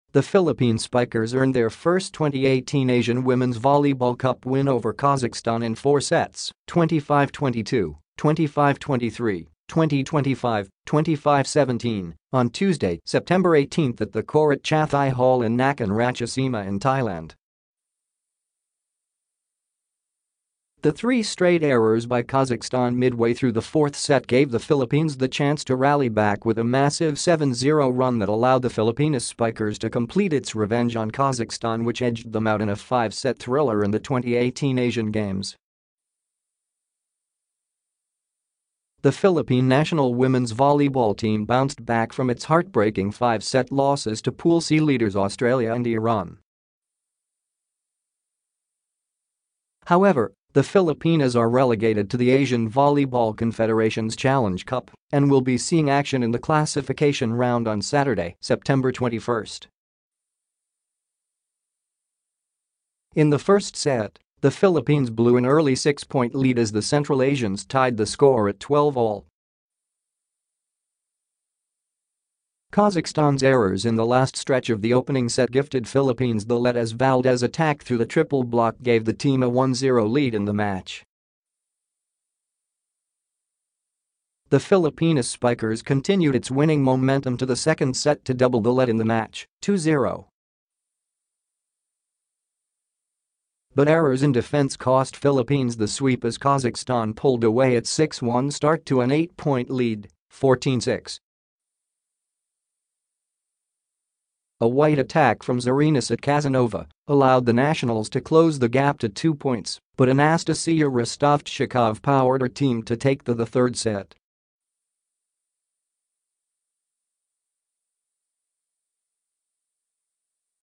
PH exacts revenge on Kazakhstan for 1st win in AVC Cup
The Philippine spikers earned their first 2018 Asian Women's Volleyball Cup win over Kazakhstan in 4 sets, 25-22, 25-23, 20-25, 25-17, on Tuesday, September 18 at the Korat Chathai Hall in Nakhon Ratchasima in Thailand.

The three straight errors by Kazakhstan midway through the 4th set gave the Philippines the chance to rally back with a massive 7-0 run that allowed the Filipina spikers to complete its revenge on Kazakhstan which edged them out in a 5-set thriller in the 2018 Asian Games.

The Philippine national women's volleyball team bounced back from its heartbreaking 5-set losses to Pool C leaders Australia and Iran. However, the Filipinas are relegated to the Asian Volleyball Confederation's Challenge Cup and will be seeing action in the classification round on Saturday, September 21.

In the first set, the Philippines blew an early 6-point lead as the Central Asians tied the score at 12-all. Kazakhstan's errors in the last stretch of the opening set gifted Philippines the lead as Valdez' attack through the triple block gave the team a 1-0 lead in the match.

The Filipina spikers continued its winning momentum to the second set to double the lead in the match, 2-0.

But errors in defense cost Philippines the sweep as Kazakhstan pulled away its 6-1 start to an 8-point lead, 14-6. A wide attack from Zarina Sitkazinova allowed the Nationals to close the gap to two points, but Anastassiya Rostovchshikov powered her team to take the the third set.